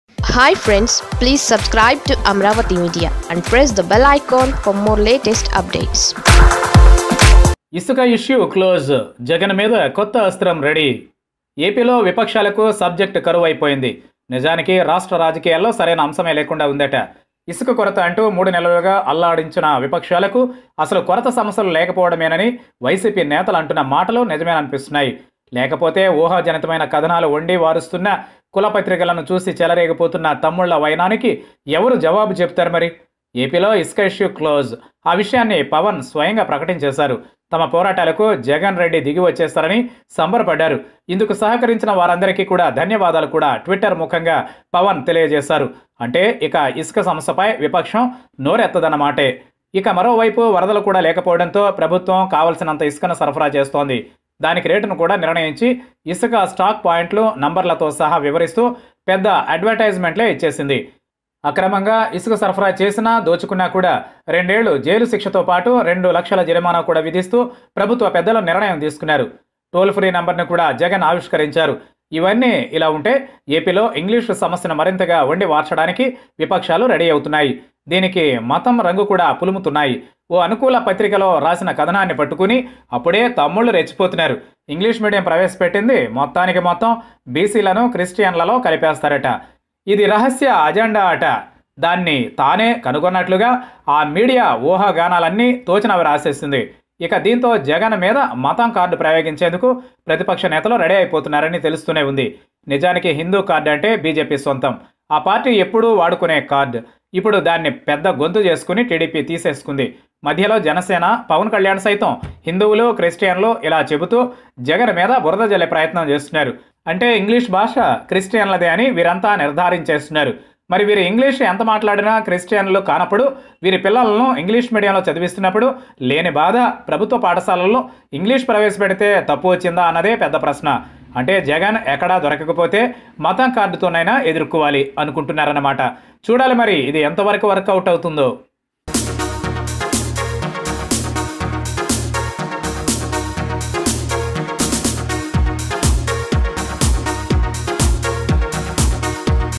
ఇసుక ఇష్యూ క్లోజ్..జగన్ పై టీడీపీ,జనసేన అస్త్రం ఇదే| Chandrababu and Pawan's New Target on CM Jagan
The problem of sand shortage is over in the AP. Now the TDP and Janasena are looking for a new problem to target the chief minister YS Jagan Mohan Reddy. Only one thing remains for them to fight against the YCP Government.
Watch this video to know that.